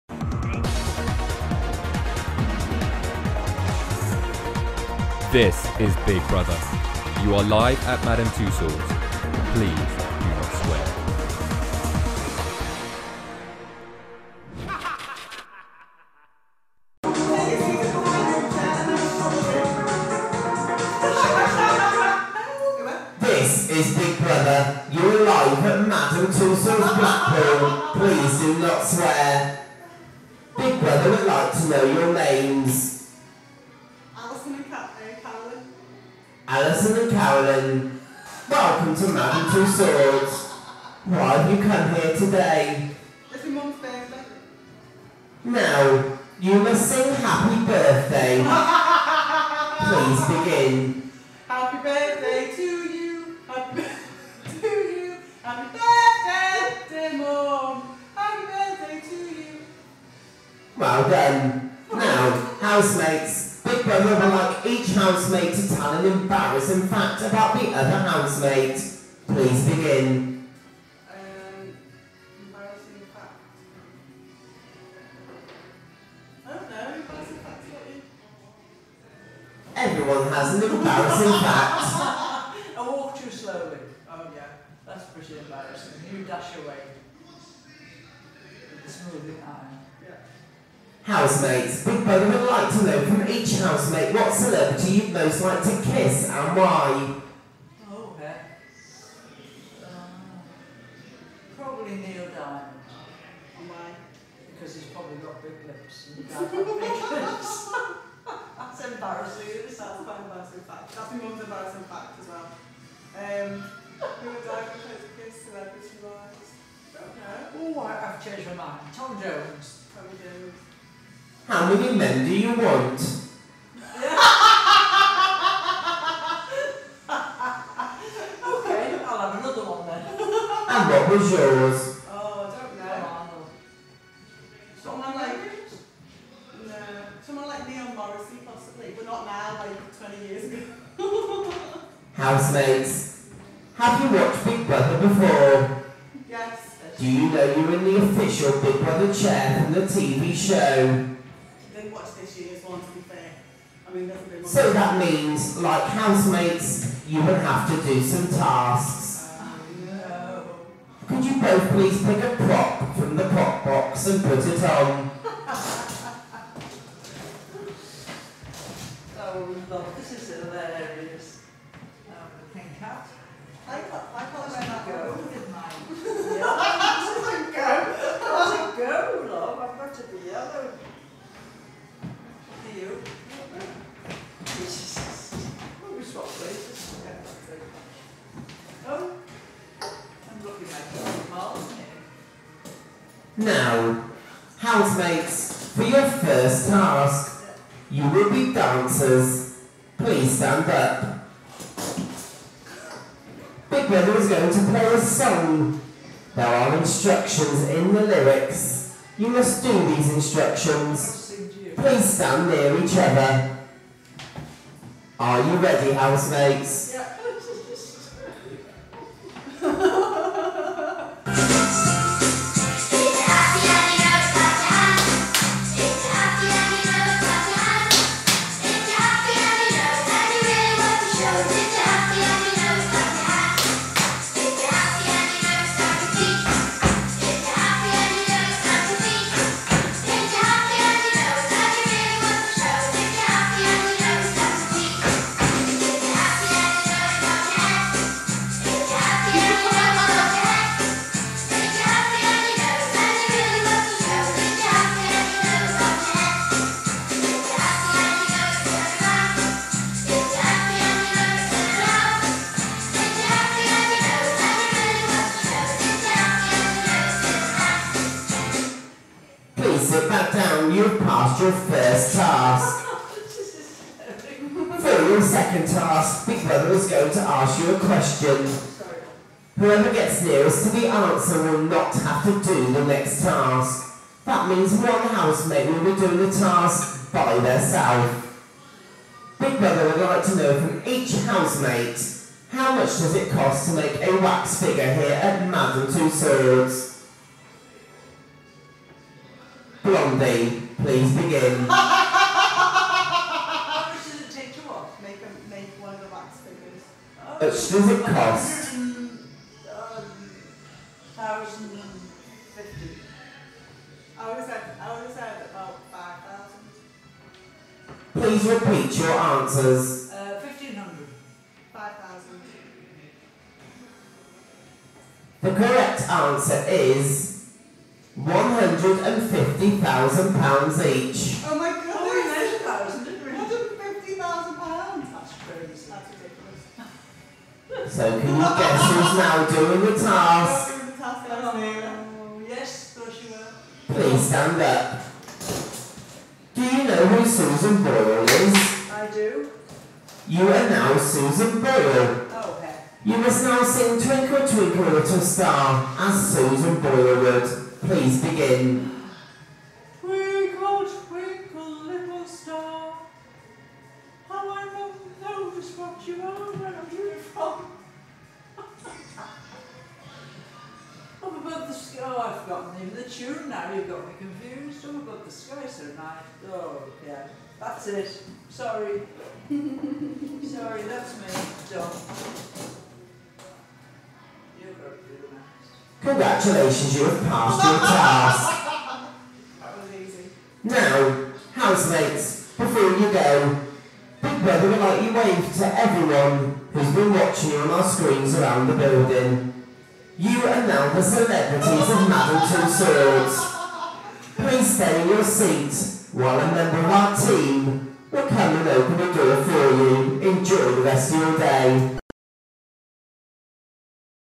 Big Brother
Me & Mum in the Big Brother Diary Room in Madame Tussauds on her 65th birthday trip to Blackpool in 2011.